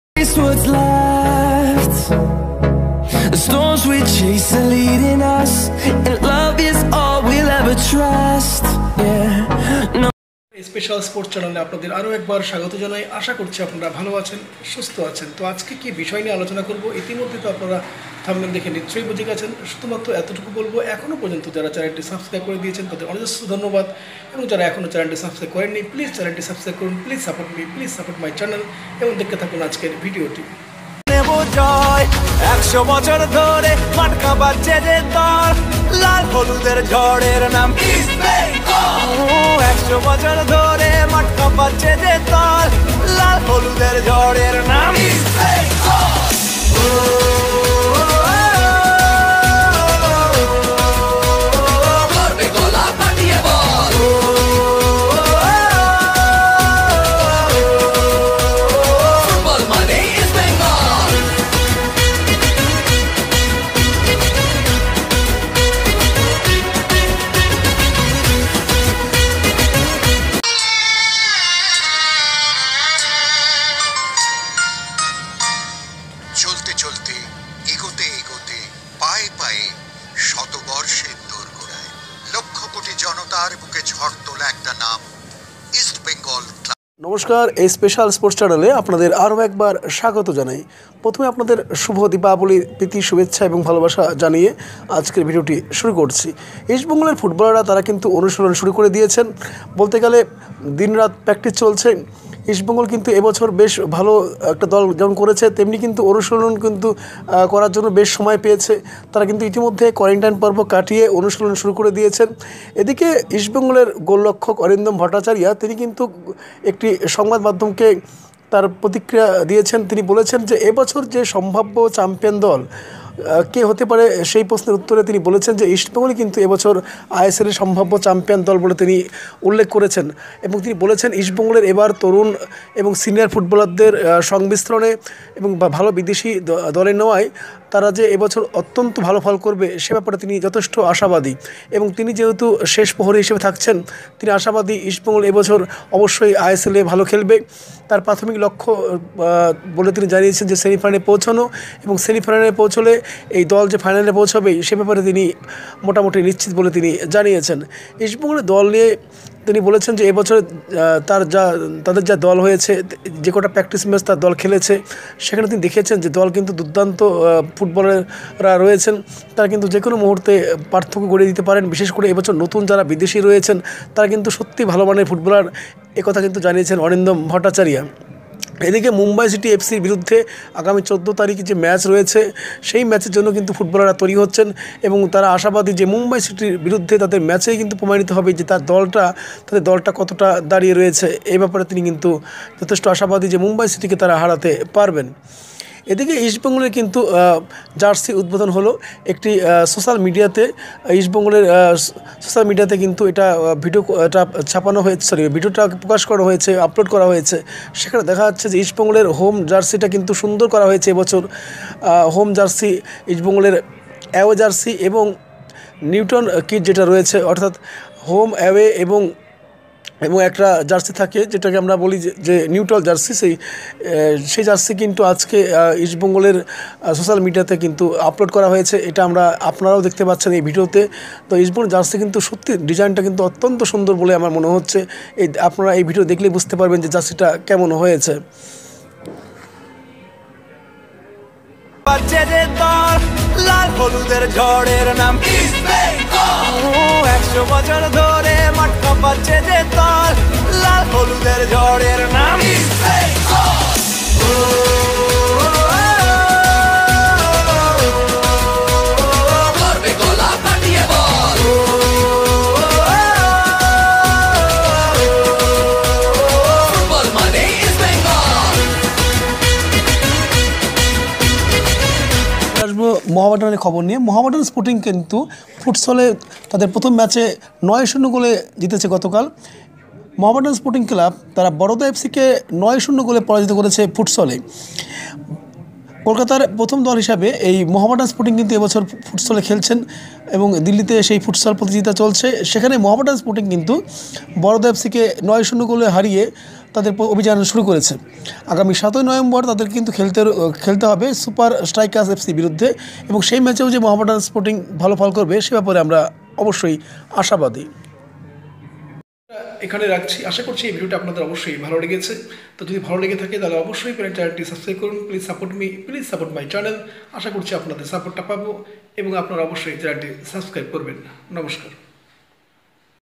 অরিন্দম ভট্টাচার্যের মতে আইএসত্রল জিতবে কোন দল?ইস্টবেঙ্গলের নতুন জার্সির উদ্বোধন ।কী খবর মহমেডানের?
ইস্টবেঙ্গল নিয়ে কী বললেন অরিন্দম ভট্টাচার্য ।ইস্টবেঙ্গলের নতুন জার্সির উদ্বোধন হল ।কী আপটেড বেরিয়ে আসছে মহমেডানের?